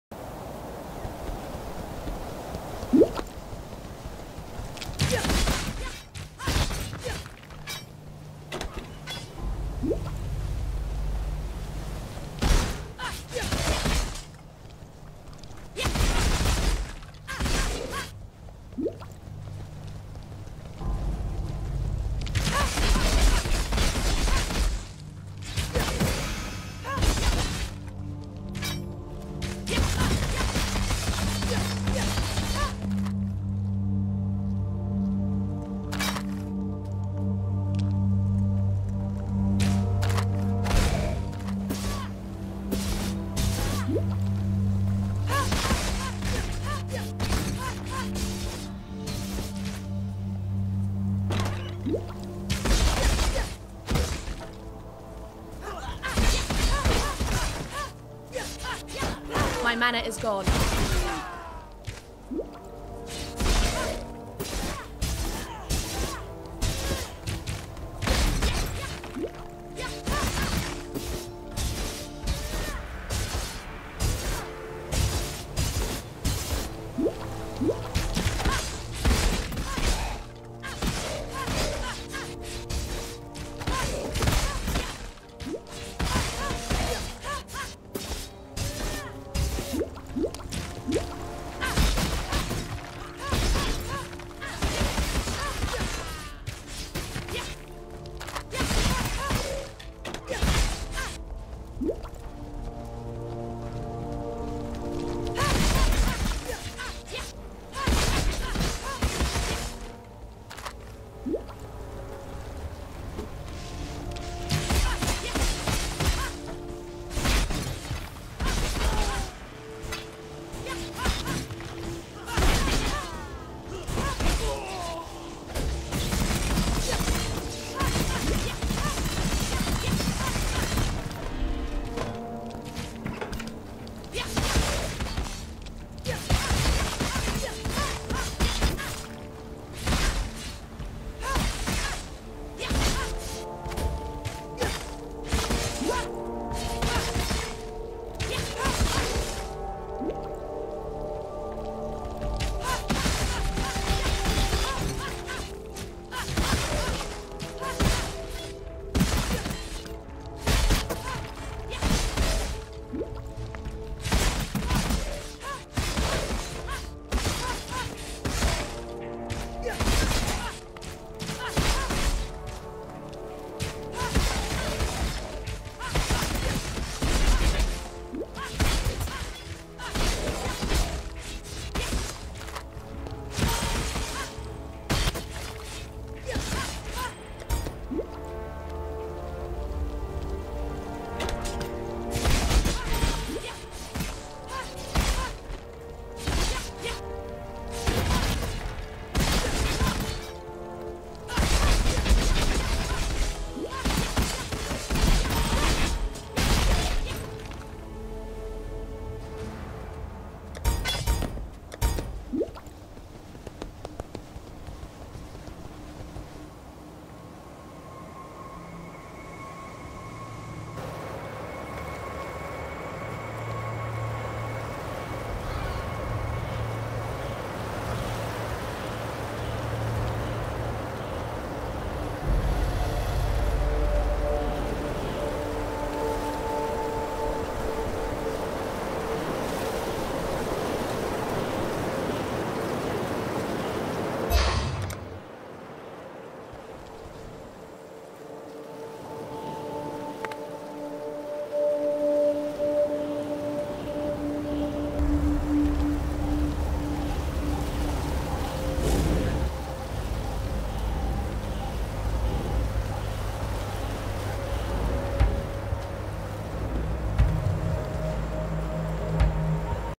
Cruel Ledge farming with OP gear
I just bought some gems and 4-linked Double Strike, and just started using Mightflay. It's pretty cool and effective. While leveling you can buy some leveling uniques for 1-2 alchs, they help a lot.